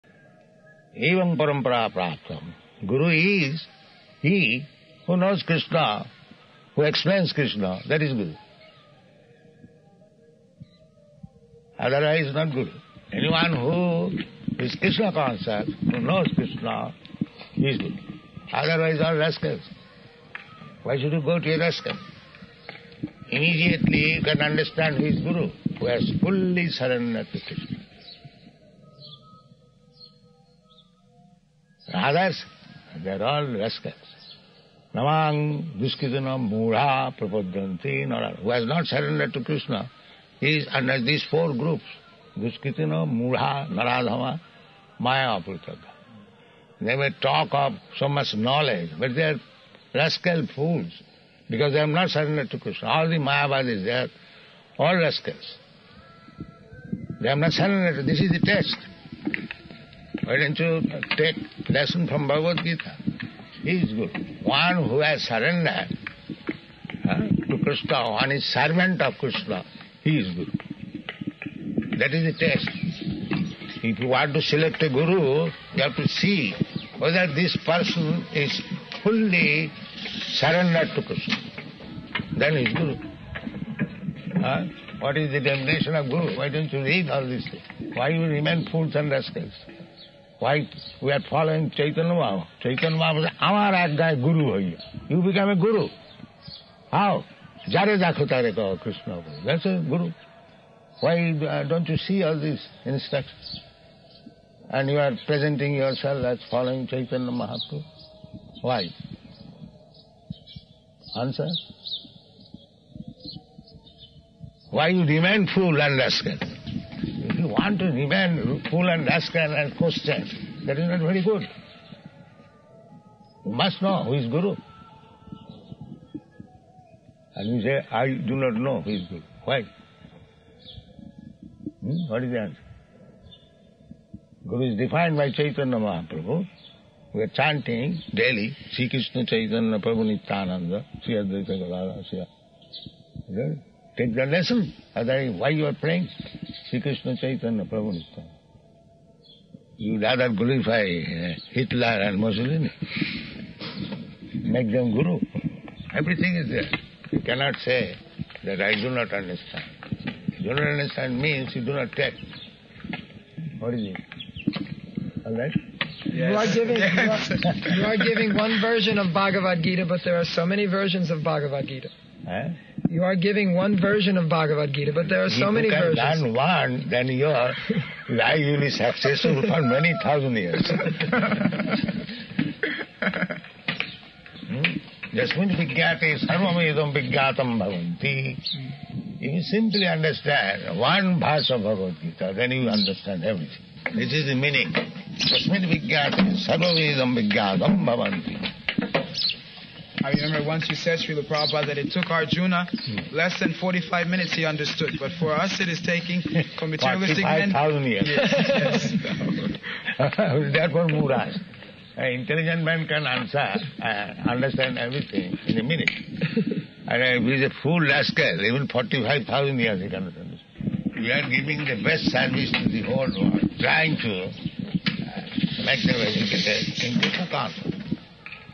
You Must Know Who Is Guru ‼
Know who is a true guru! Srila Prabhupada explains in plain words.